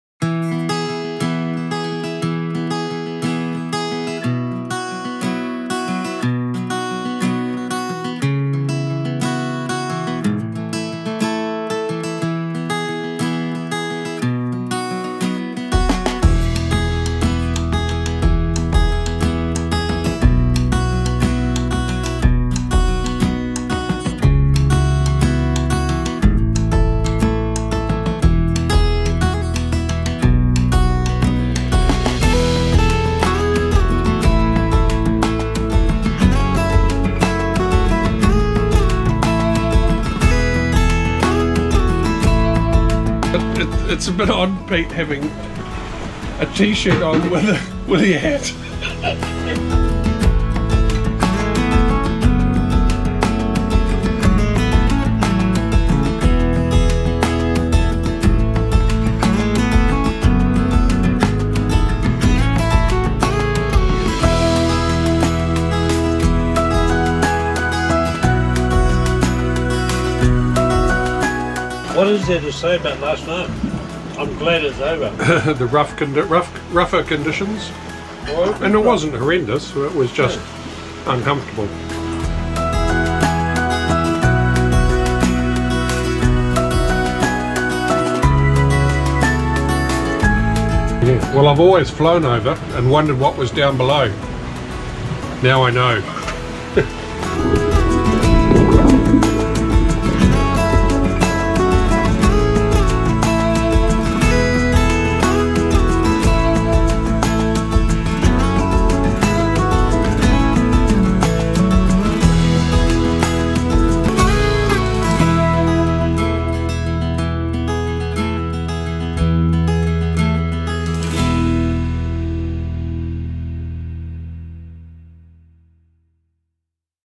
Tamariki's Voyage Hobart to Picton (Preview)
Undertaken in May 2019 Peter Mortimer and I sailed the yacht Tamariki, a 45ft Gary Wheeler designed Ketch from Hobart Tasmania to Picton New Zealand. This is the short of the documentary that will cover the trip.